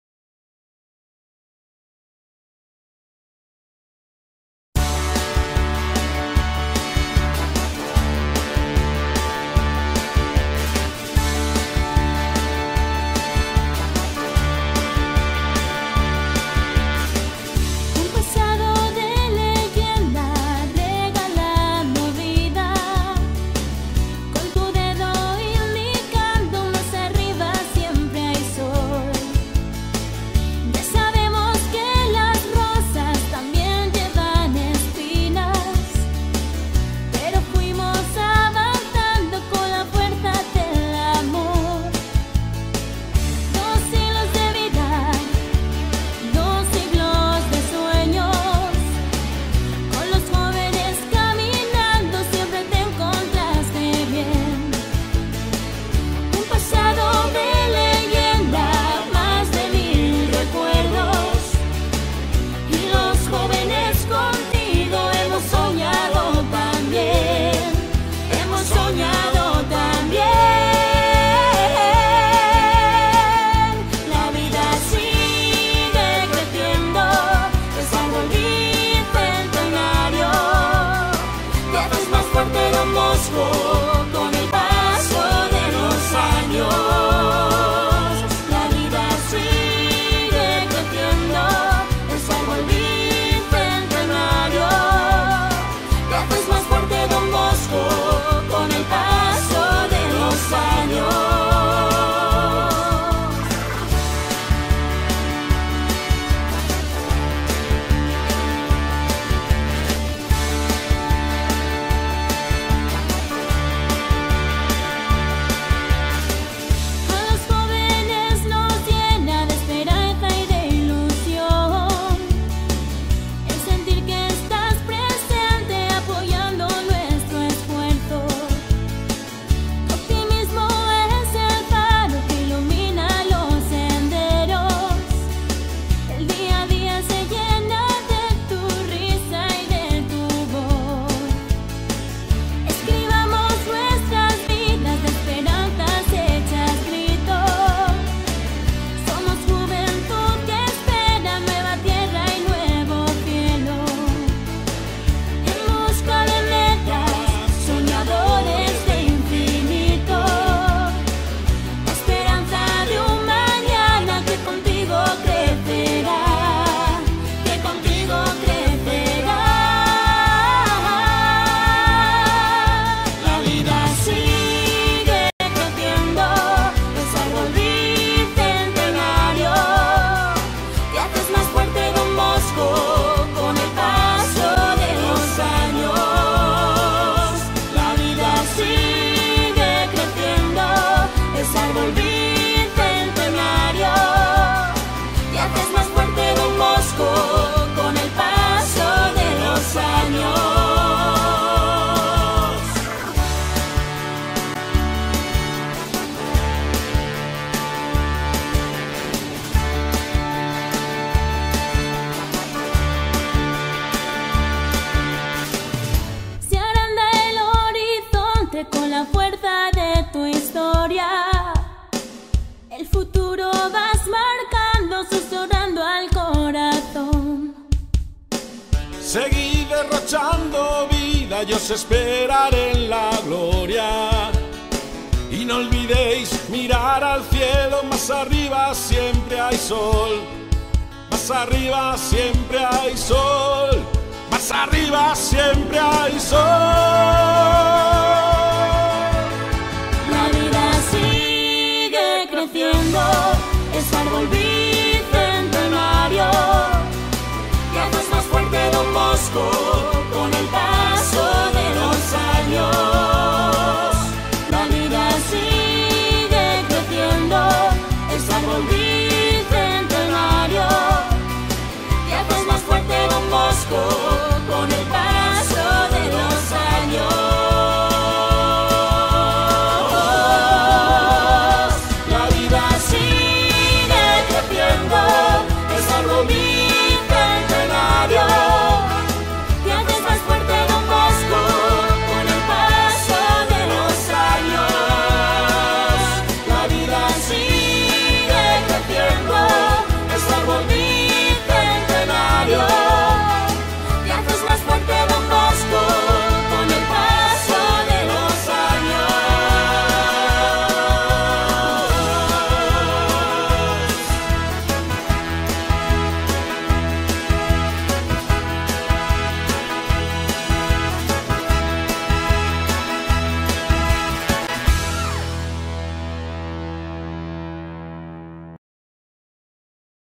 Erección canónica San José del Valle
Video con fotos históricas de la vida del centro local.